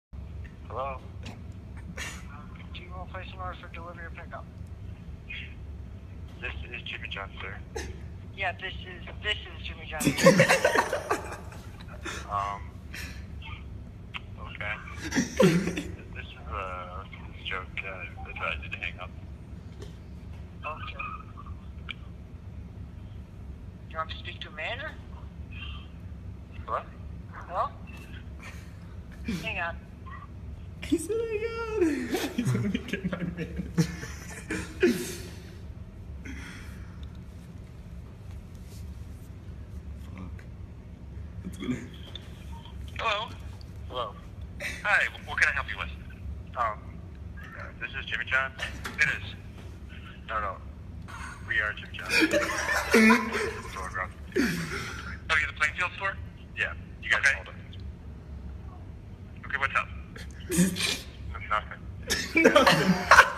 Jokester Connects Two Take-Out Restaurants on Conference Call. Hilarity ensues.
Hello, this Jimmy John's.